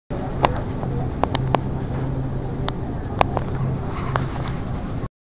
Gangstalked at supermarket Jan 04, 2015 - 10AM
The guy in the ball cap is a white caucasian male, 40 + years of age. Was following me around the store pretending to look for something.